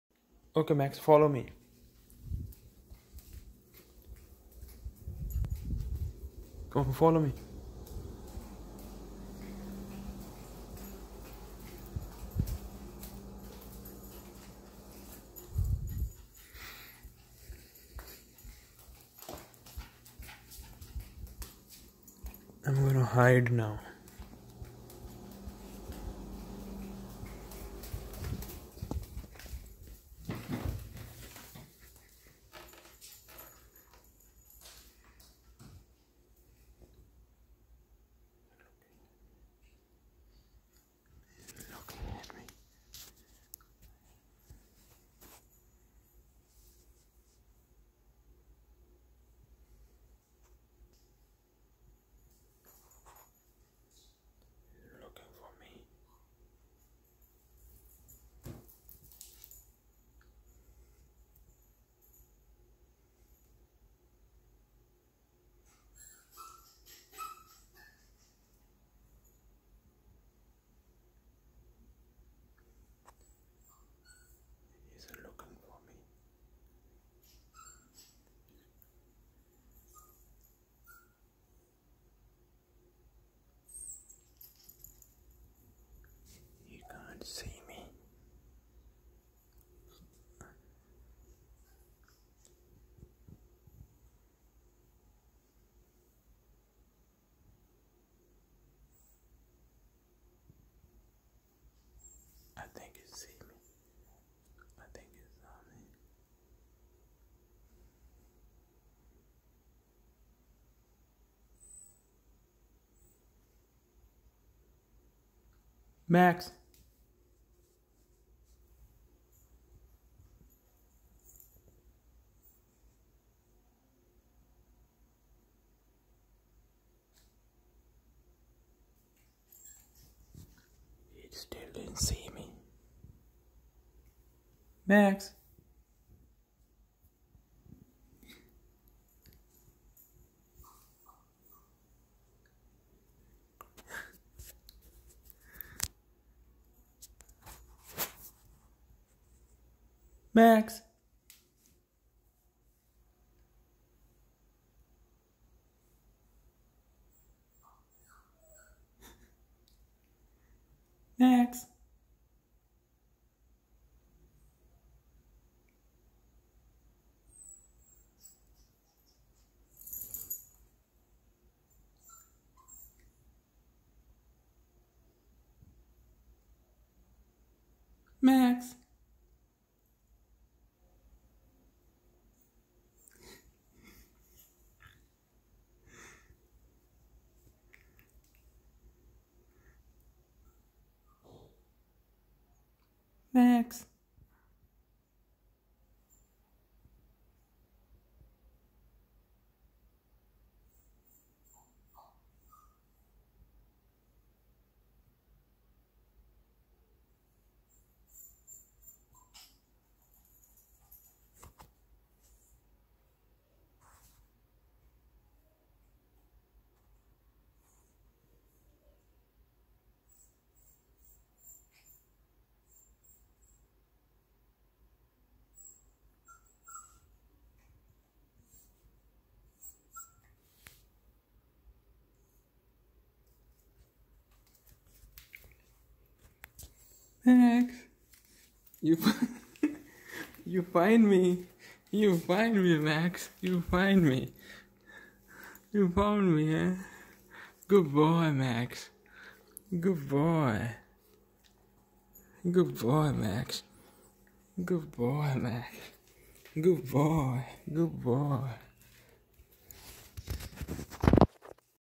Max and me playing hide and seek.
Hide and seek at night.